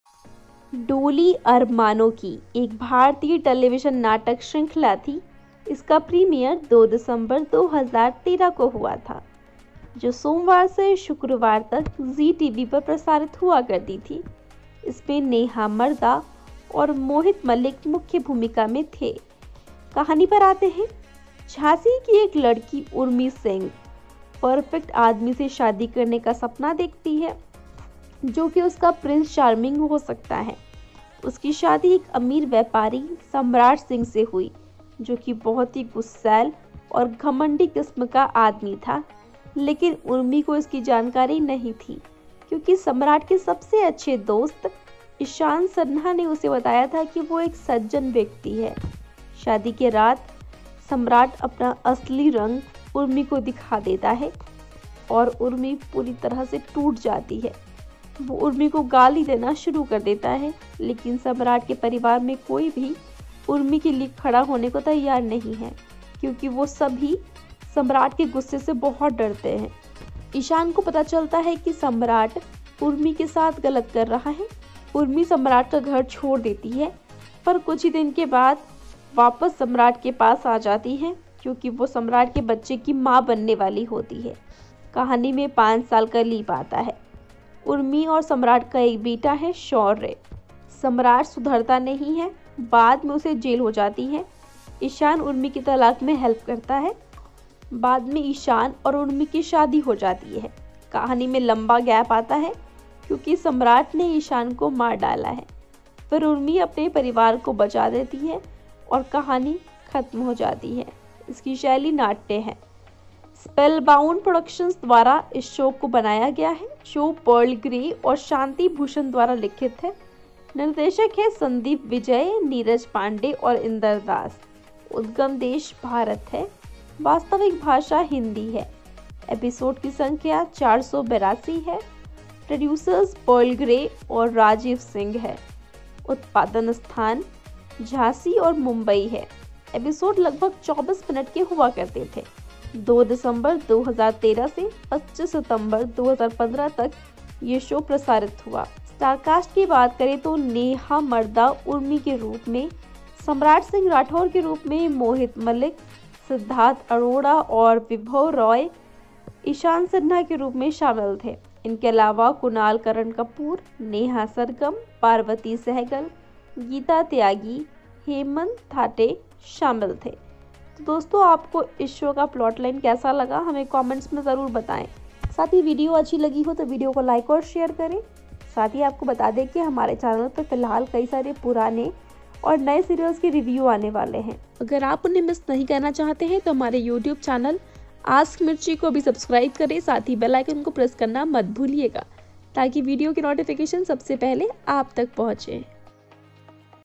Doli Armaano Ki Serial Review| Zee TV| Siddharth Arora| Snigdha Srivastava| Geeta Tyagi
Doli Armano Ki or “The Palanquin of Dreams” is an Indian drama and romance TV series from Zee TV. It was aired last December 2, 2013 and running up to the present. The show was under the Pearl Grey of Spellbound’s Production. Doli Armano Ki is evolving on Urmi’s dream and her anticipations from what her husband will be. She marries Samrat Singh Rathore (played by Mohit Malik) who is totally her opposite.